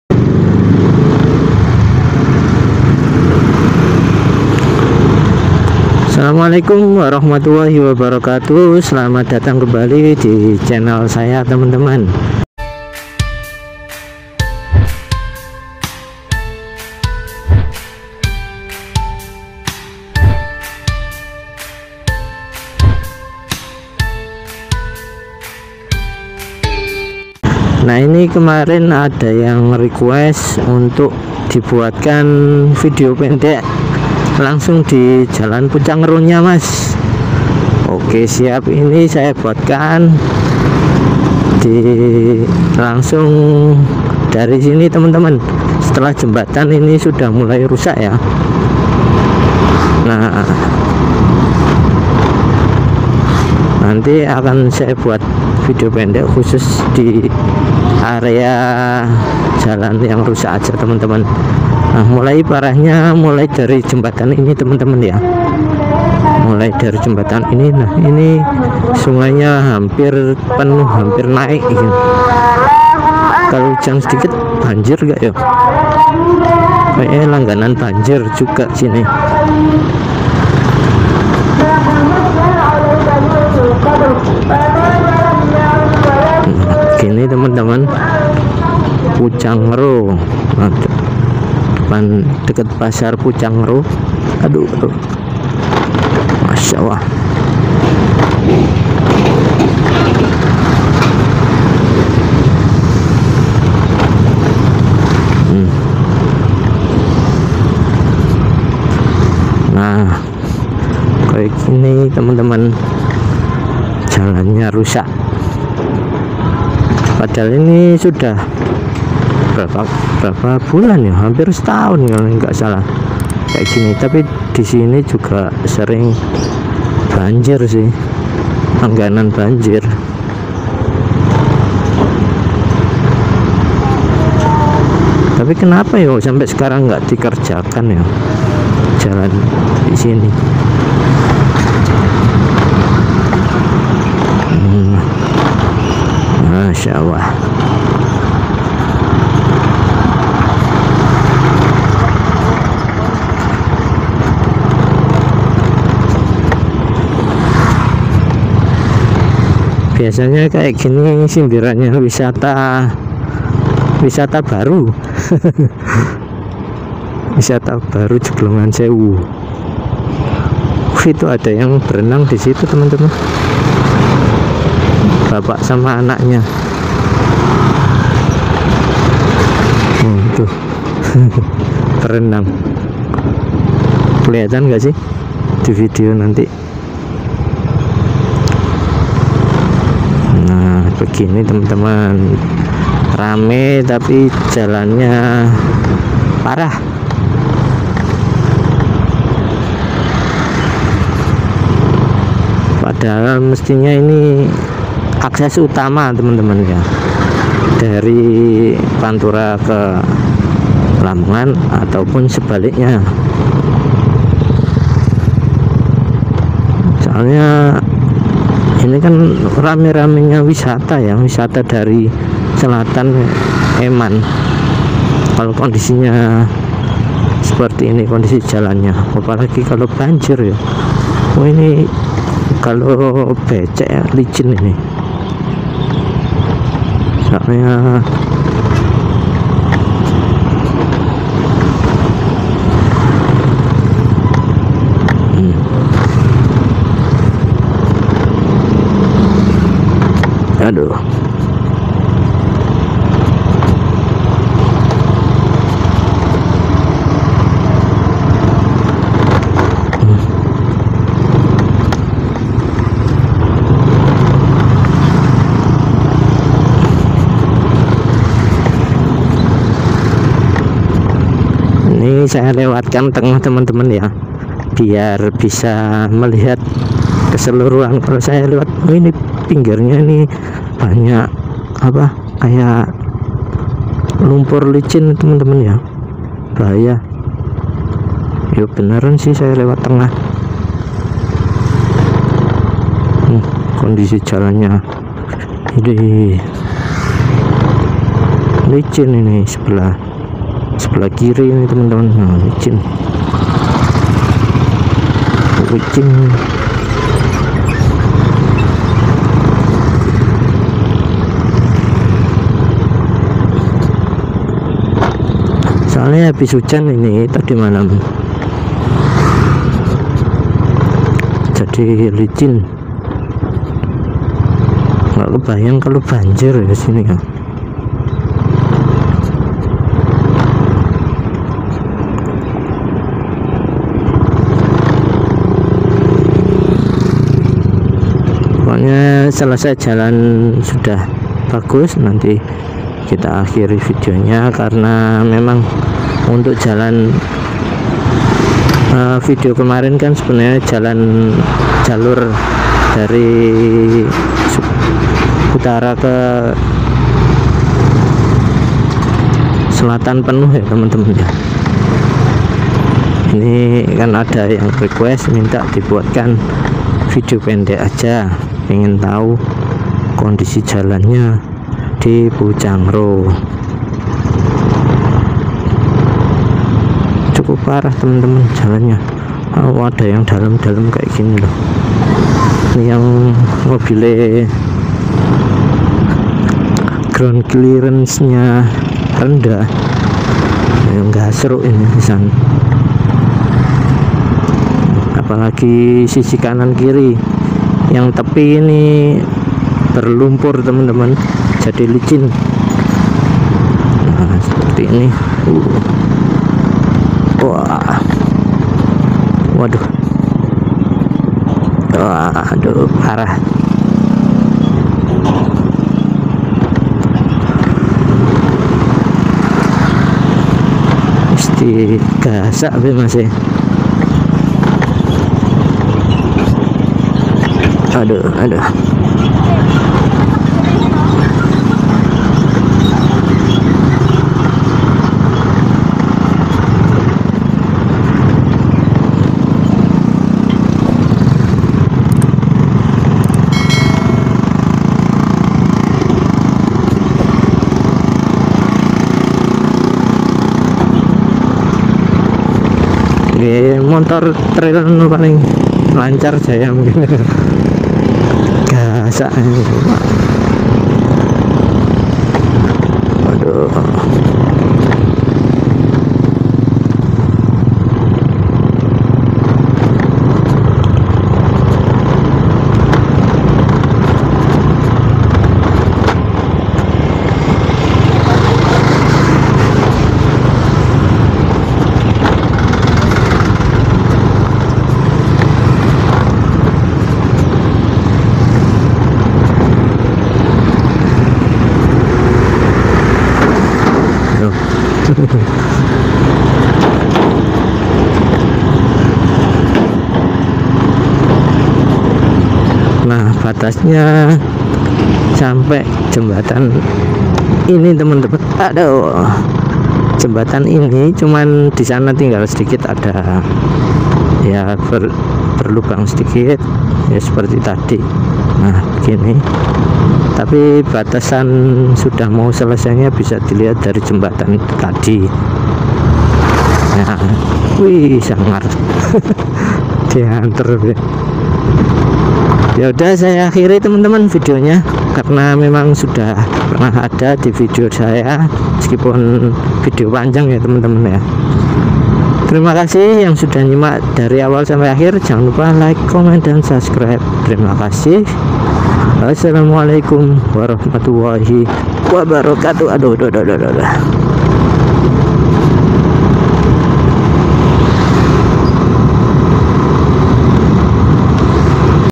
Kondisi Jalan Raya Pucangro Lamongan Terbaru ~ ELHA Channel
Kondisi Jalan Raya Pucangro Lamongan Terbaru hari ini, video ini diambil pada tanggal 23 Mei 2022. jalan pucangro lamongan ini sudah lama mengalami kerusakan akan tetapi belum tindakan perbaikan jalan, jalan ini merupakan jalan utama akses menuju lamongan pantura, mengingat di lamongan utara ada lokasi wisata yang ramai dikunjungi, jadi sangat disayangkan sekali.
diharapkan dinas terkait segera menangani perbaikan jalan di pucangro lamongan ini.

Terus dukung channel ini dengan cara:
Terima kasih!
Gear:
📷 : Pocophone, Akaso V50X
🎤 : Boya BY M1
🛠️ : Kinemaster
🛵 : Vario 125 2012
📌 : Lamongan